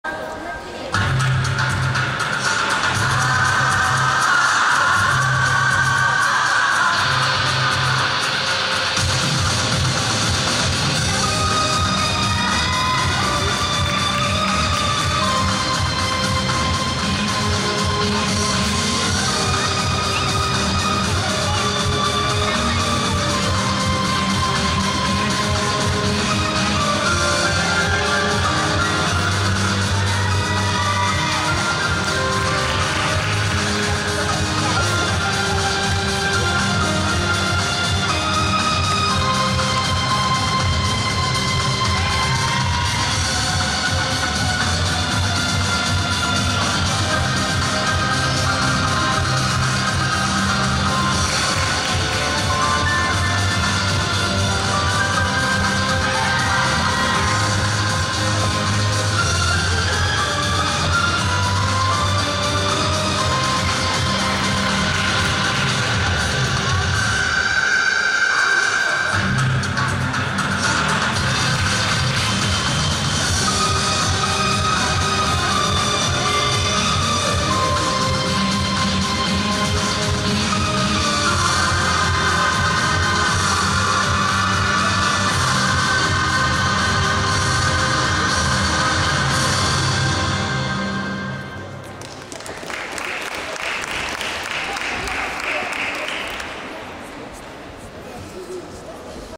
Чемпионат России по художественной гимнастике - 2011(47)
Пенза, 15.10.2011. Многоборье. Захарова Юлия, мяч.